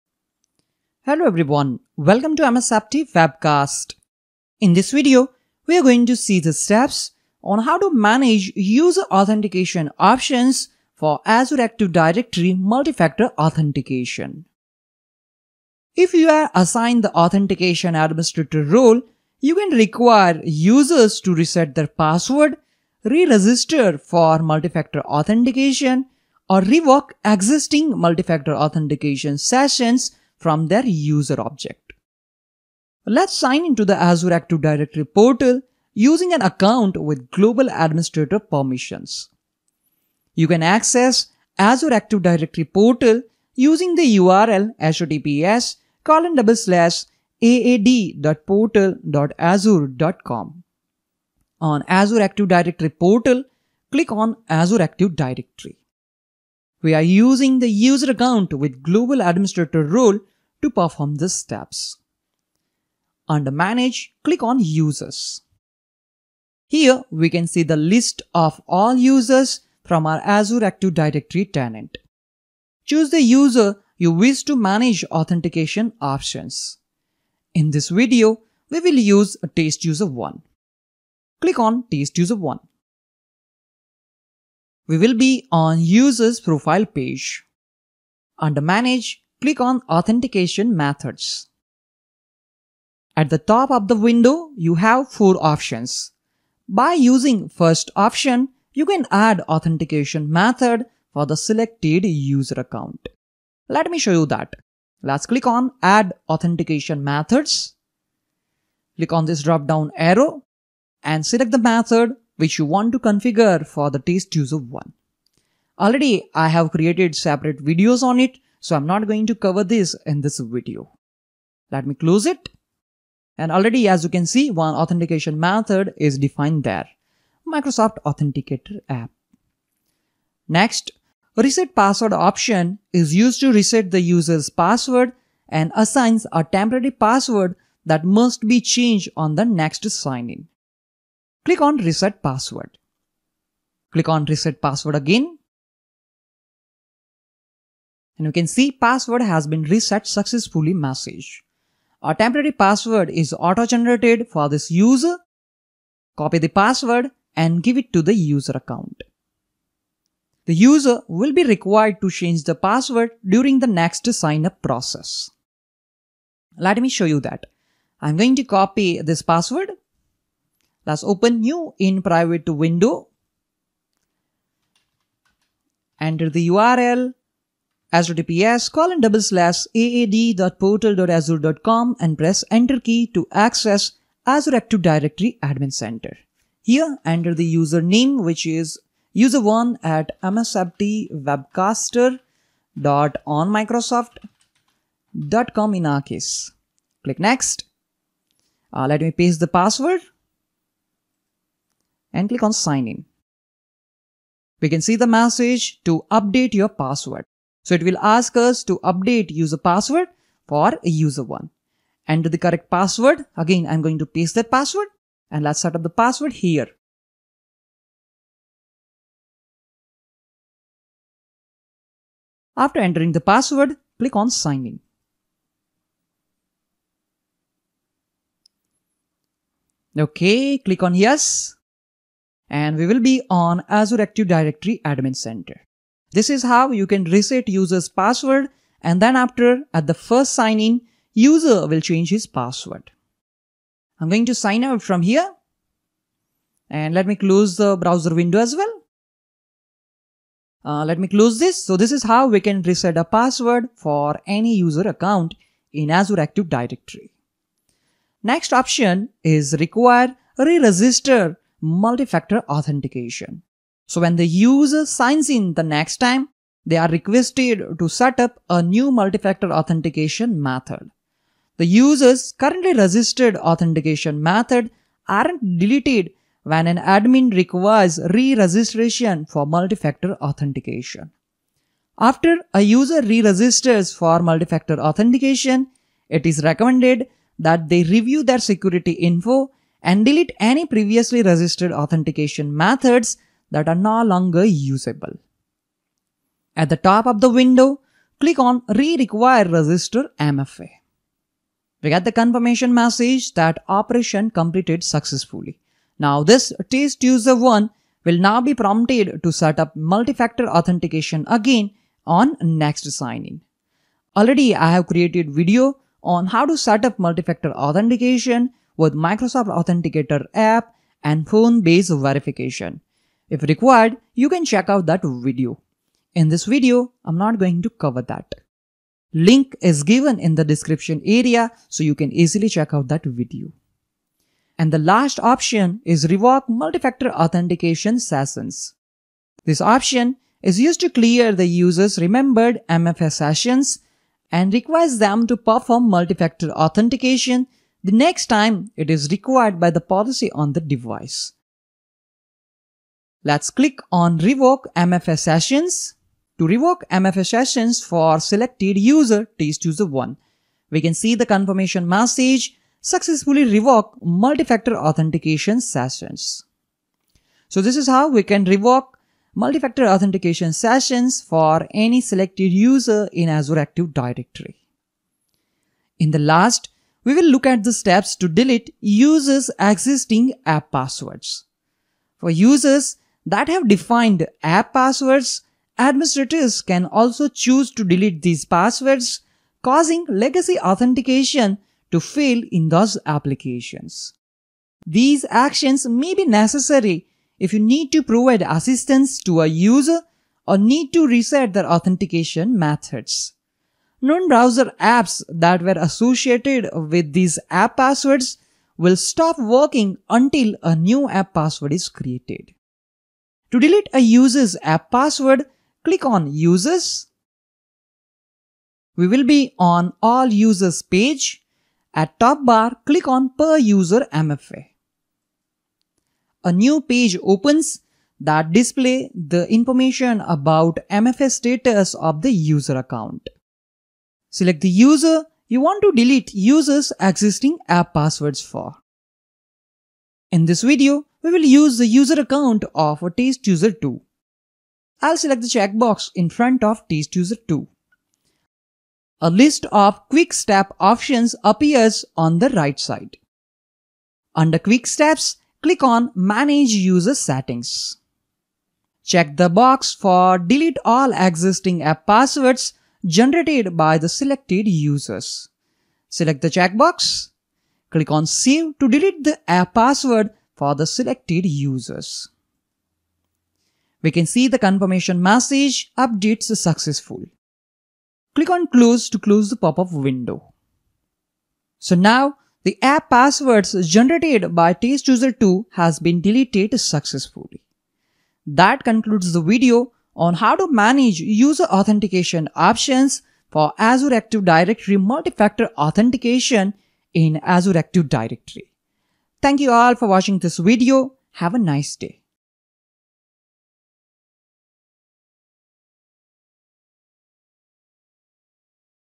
34. Manage User Authentication Options for Azure AD MFA
Microsoft Azure Active Directory Beginners Video Tutorials Series:
This is a step by step guide on How to Manage User Authentication Options for Azure AD User Account using Azure AD Admin Center web interface.